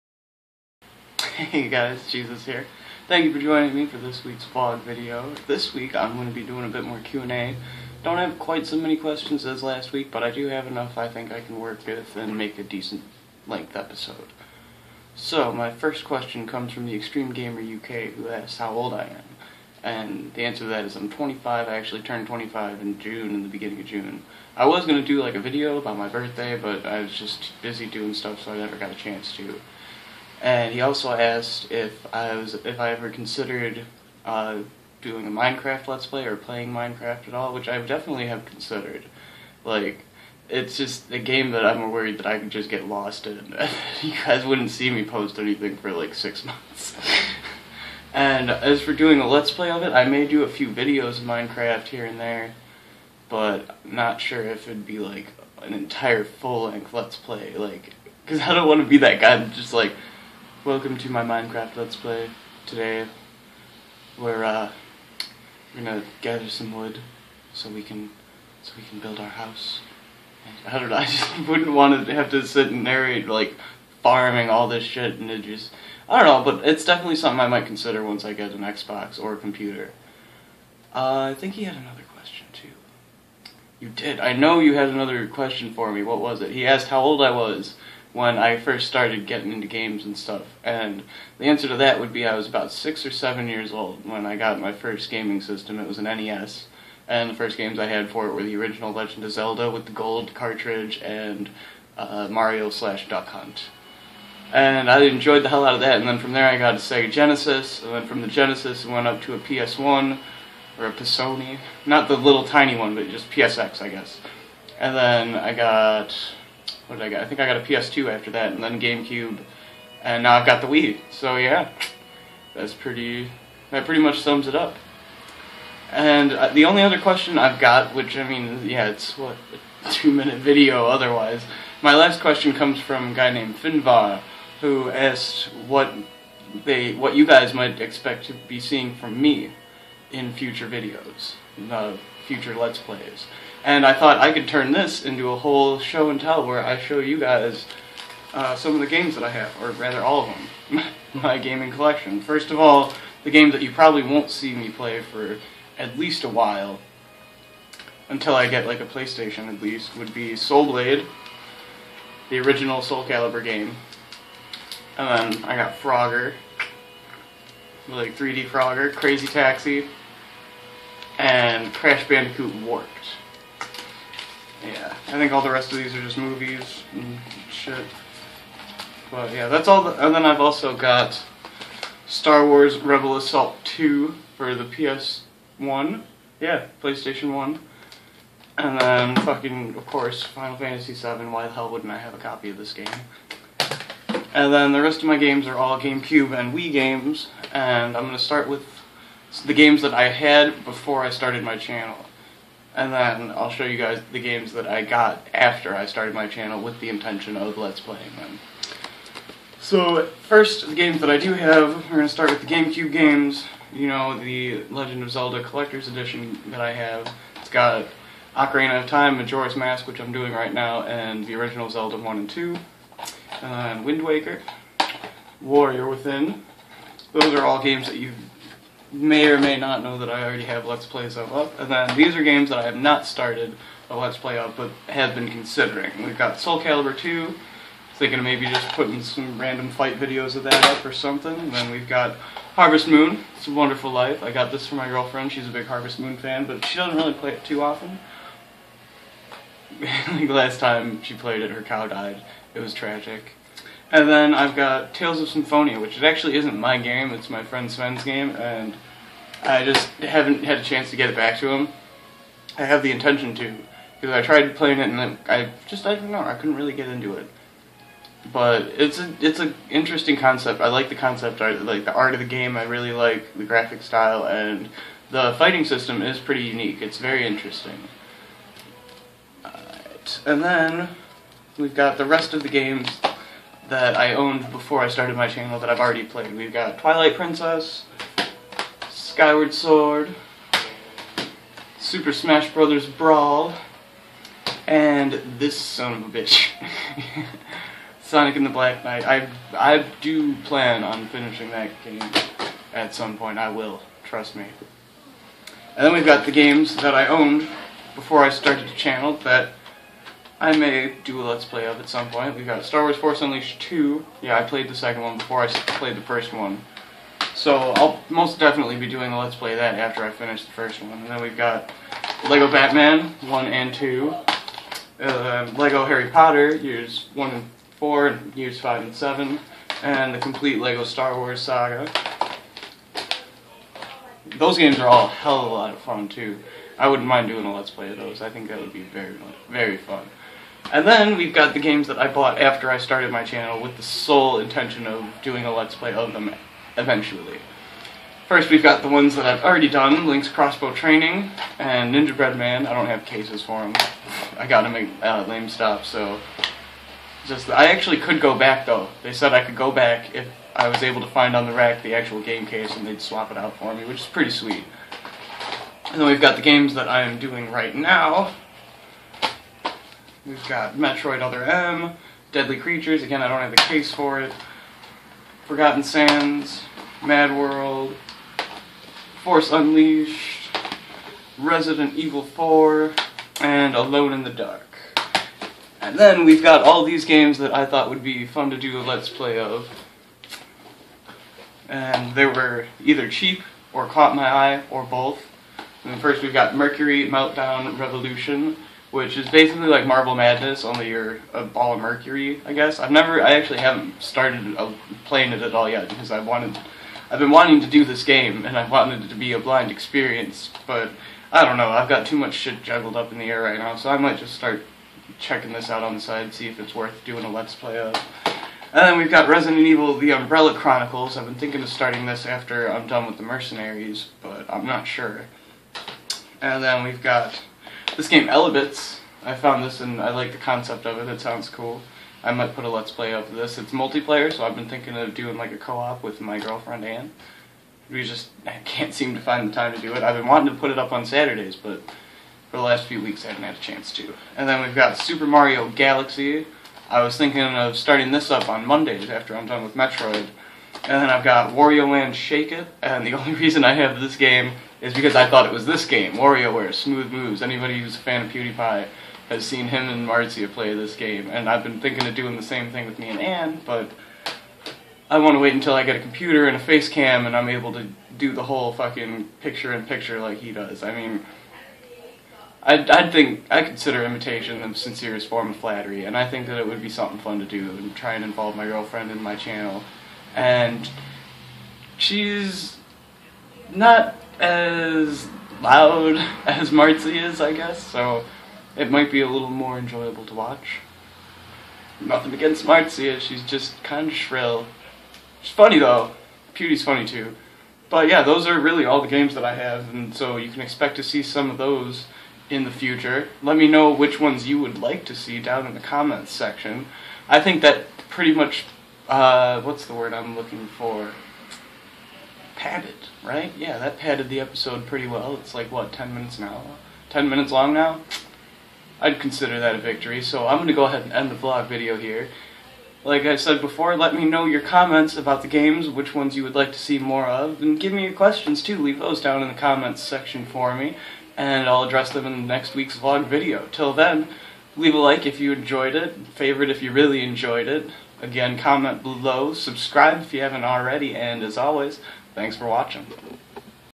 My Game Collection!
New vlog for ya! A bit more Q&A, plus my game collection. Leave a comment letting me know which games you would like to see in the future. Also, leave your questions so I can answer them in next week's vlog!